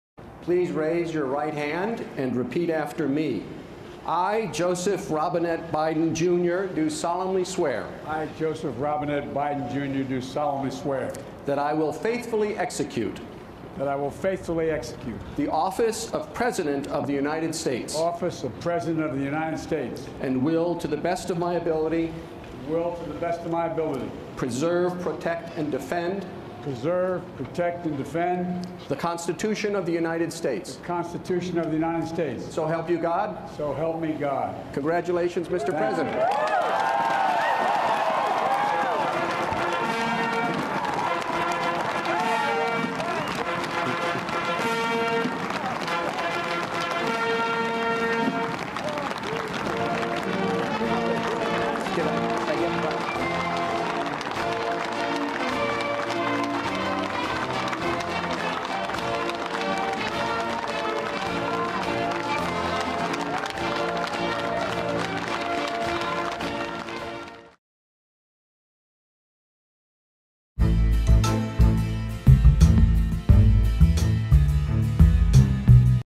بايدن يؤدي اليمين رئيسًا للولايات المتحدة أمام رئيس المحكمة الدستورية العليا
إعداد بيشوي ماهر
للاشتراك في قناة «المصري اليوم» على يوتيوب: